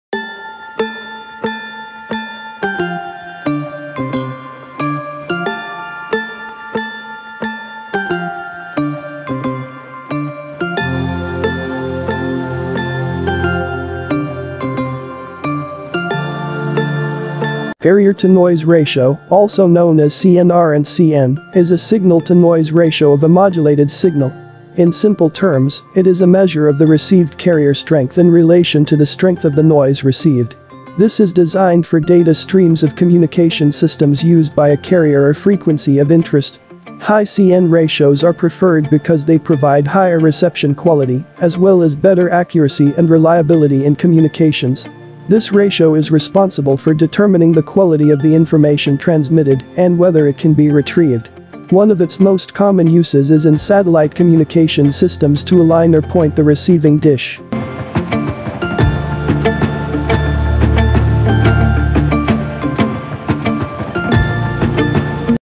CNR Full Form || What is CNR ?
Carrier to noise ratio, also known as CNR and C/N is a signal-to-noise ratio of a modulated signal. In simple terms, it is a measure of the received carrier strength in relation to the strength of the noise received. This is designed for data streams of communication systems used by a carrier or frequency of interest. High C/N ratios are preferred because they provide higher reception quality as well as better accuracy and reliability in communications. This ratio is responsible for determining the quality of the information transmitted and whether it can be retrieved. One of its most common uses is in satellite communications systems to align or point the receiving dish.